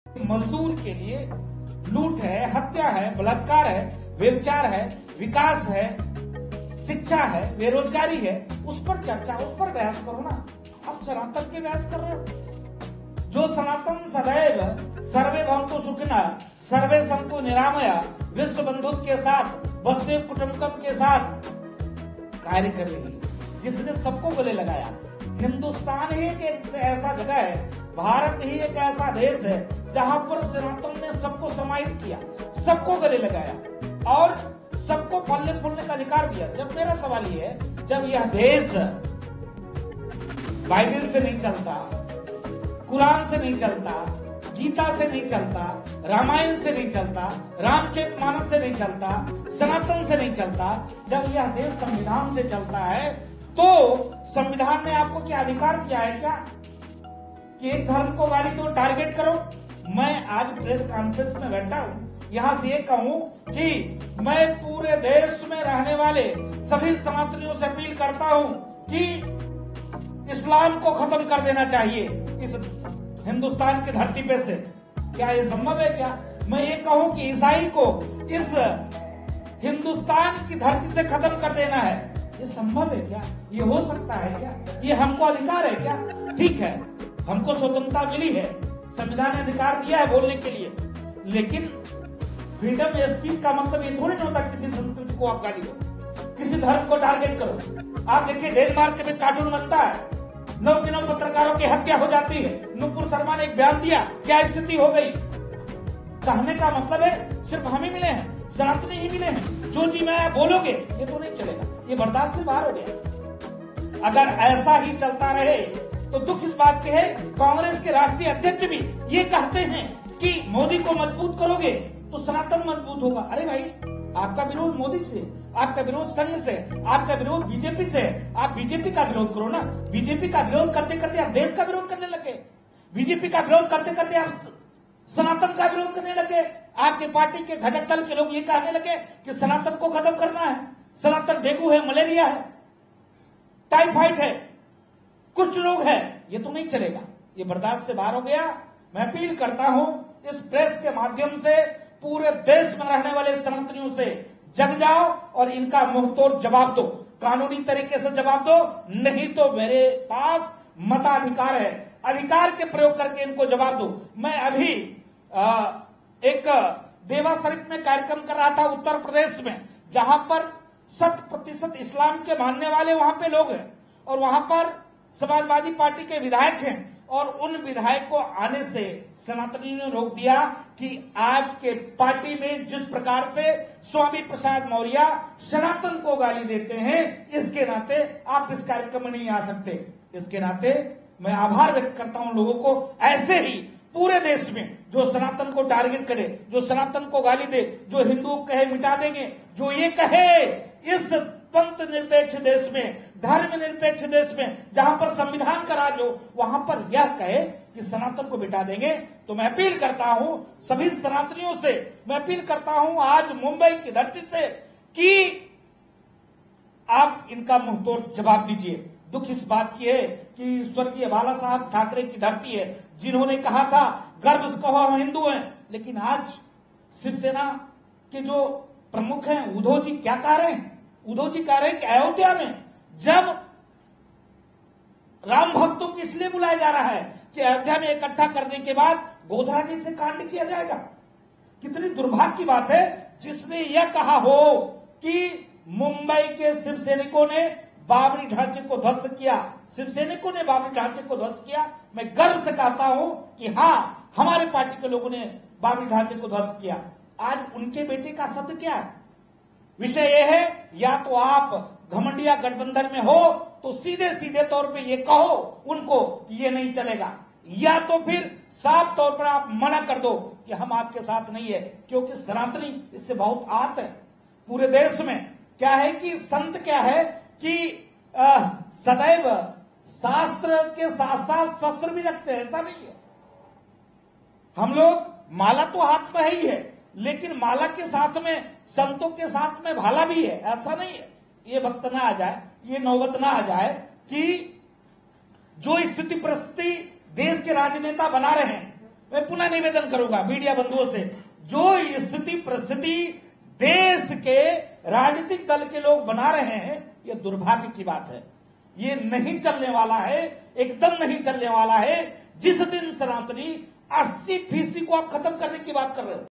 "यह देश संविधान से चलता है", रामायण और सनातन से नहीं Raju Das on Udayanidhi
Raju Das on Udayanidhi